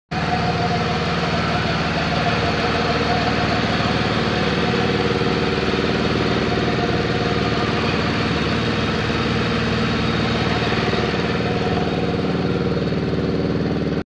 2003 Ford Crown Victoria Police Interceptor for sale at auction | bidding closes December 17, 2019
2003 Ford Crown Victoria Police Interceptor
129,069 miles on odometer
4.6L V8 SOHC 16V gas engine
Automatic transmission
AC and heat
Cruise
Power windows and locks
225/60R16 tires
ABS light on
Missouri title
2FAFP71W43X171167
It's located in Lebanon, MO, and we're selling it to the highest bidder regardless of price.